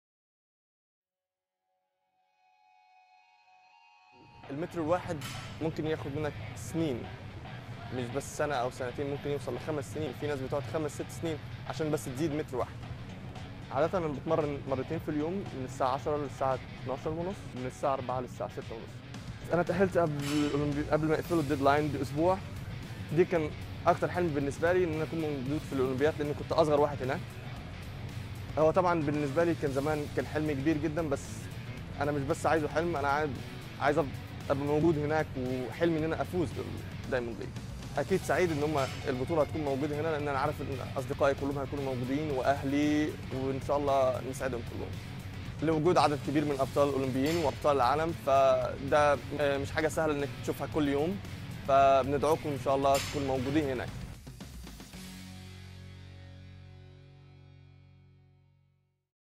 Ahmed Bedeir | Team Qatar Athletics
شوفوا بطل فريق_قطر الأولمبي أحمد ماغور يستعد للمنافسة الأولى له في الدوري الماسي لألعاب القوى!
Watch TeamQatar's Olympian @ahmed__magour as he prepares for his first @DiamondLeagueathletics appearance!